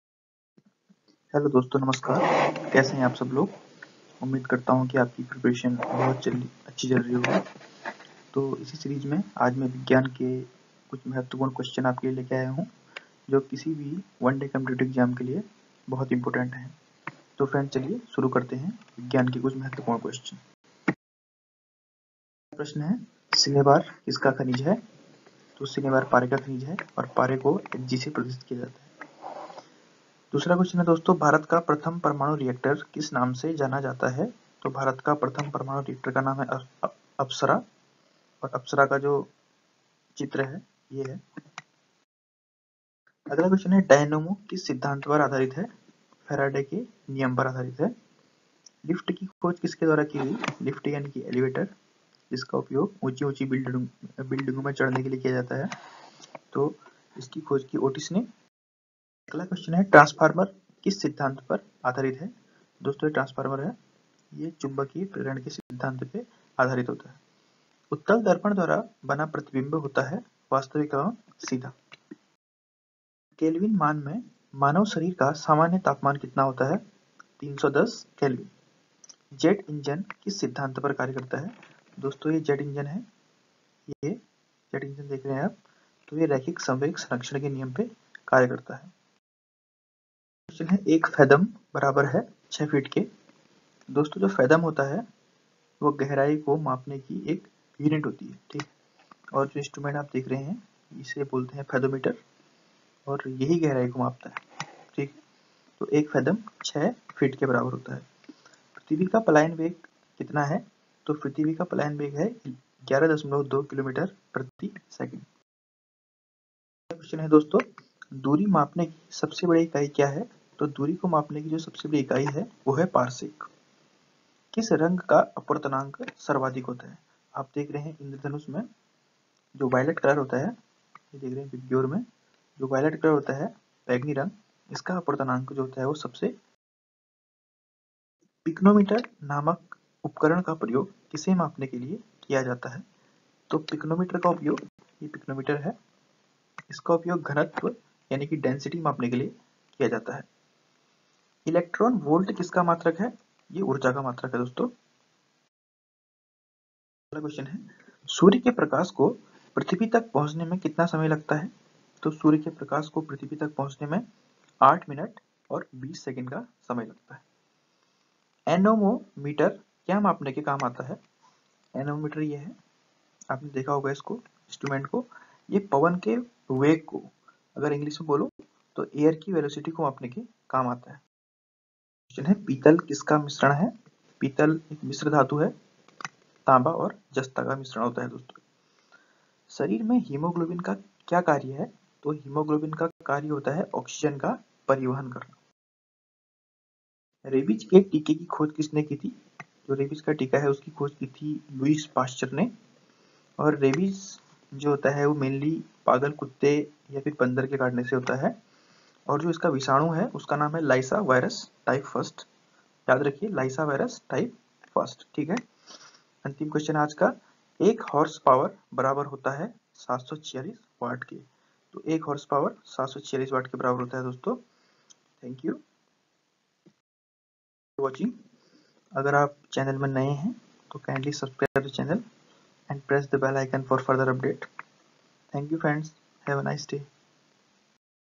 General Science Questions|| Previous Year Paper For SSC, RPF
General Science Questions|| Previous Year Paper For SSC, RPF||2019

FRIENDS IN THIS VIDEO WE ARE GOING TO DISCUSS IMPORTANT SCIENCE QUESTION FOR SSC AND VARIOUS RAILWAY EXAMS.

TO CHECK OUR PLAYLIST KINDLY VISIT LINKS GIVEN BELOW-